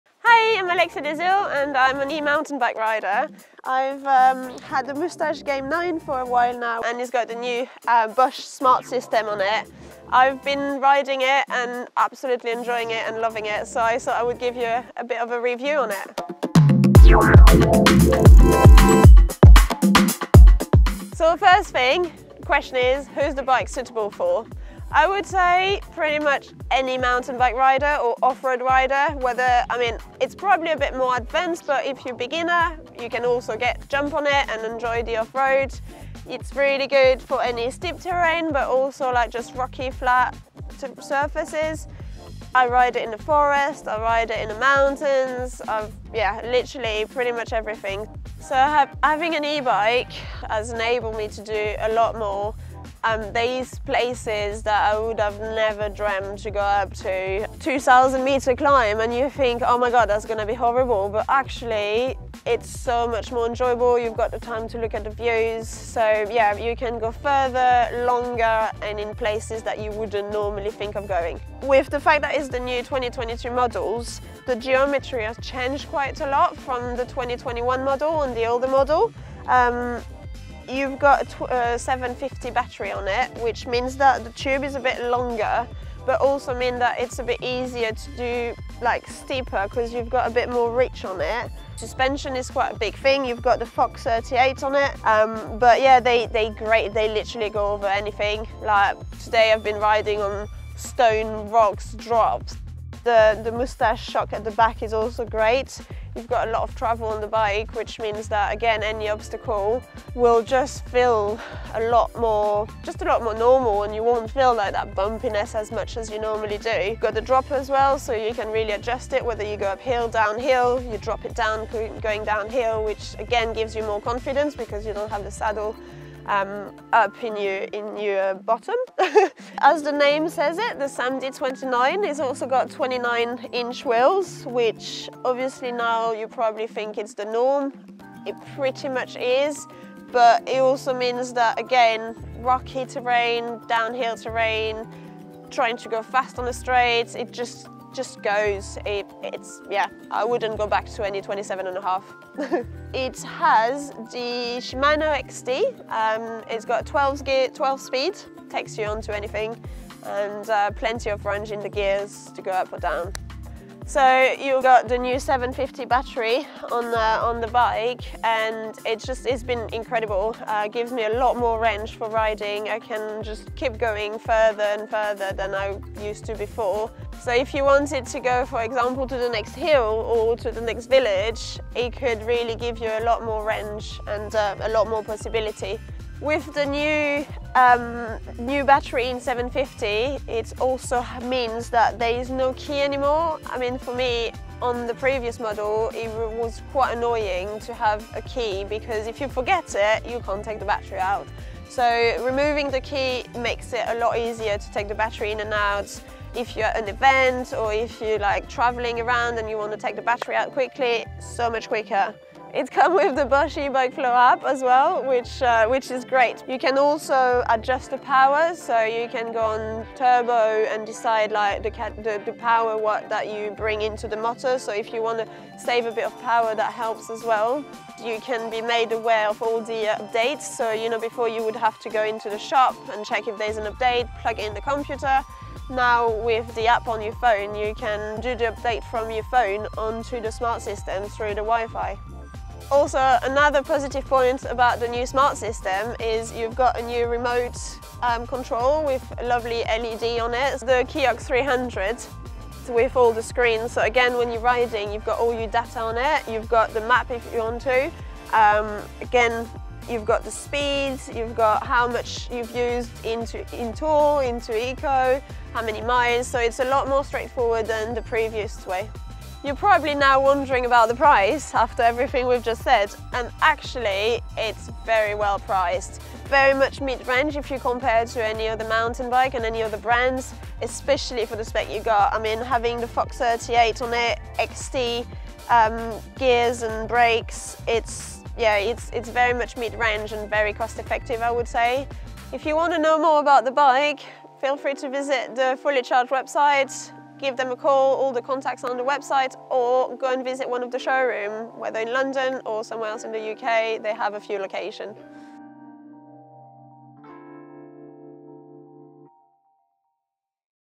We reviewed the Moustache Game 9 Electric Mountain Bike! | Fully Charged
We travelled down to Exmoor in South Somerset to visit @alexiadesile who took us around her brand new electric mountain bike, the @MoustacheBikes-SmilingMachines Game 9. To visit the Moustache Electric Mountain bike series

All of the new Trail and Game series from Moustache come complete with the all new Bosch Smart System. You can find more about the Bosch Smart System here:

With the top of the range specifications on this bike, including the Fox 38 and @RideShimano XT, the Game 9 is the perfect bike for the weekend rider.

0:00 Intro
0:21 Who's the bike for?
0:48 What's it like having an e-bike?
1:12 Geometry
1:32 Suspension
1:58 Features
2:45 New 750wh battery
3:33 New Bosch eBike Flow app
4:02 Features of the new Smart System
4:29 Price point
4:52 Where to buy?